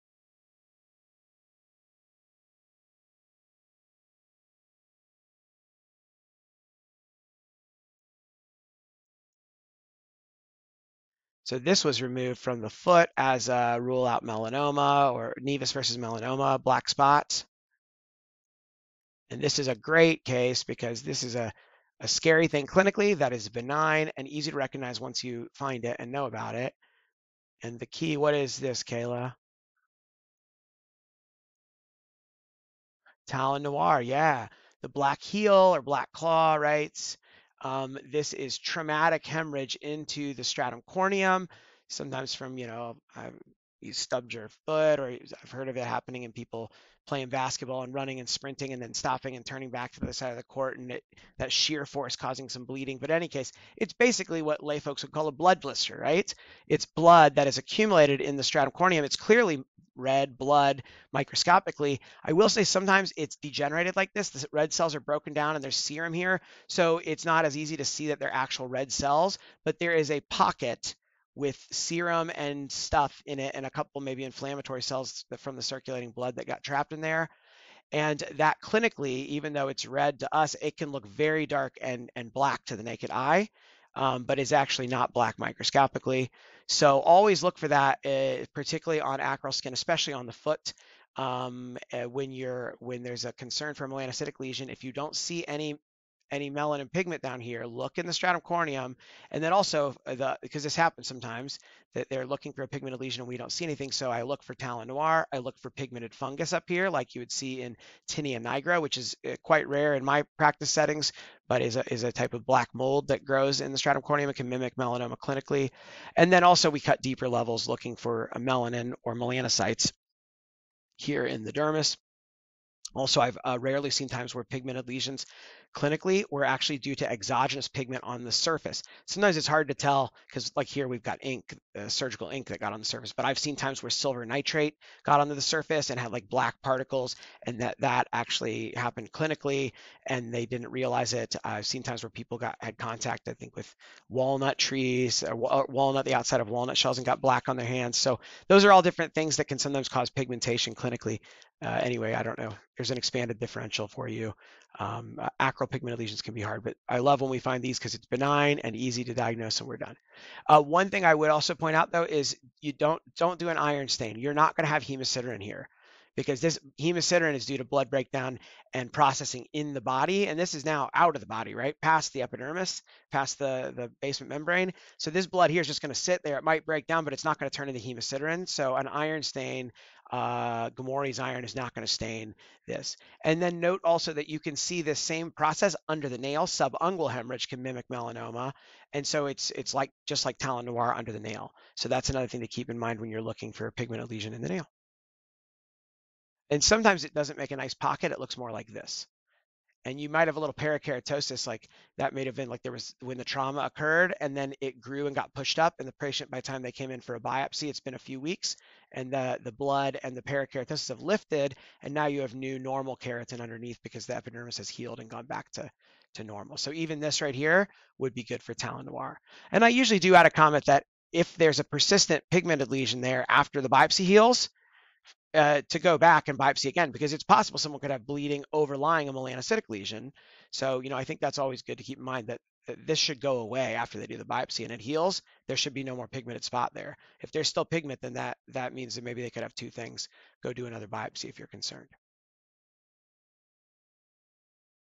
"Blood blister" under microscope Talon noir subcorneal hemorrhage black spot on foot or hand skin
From my melanocytic dermatology/pathology board review. Enjoy!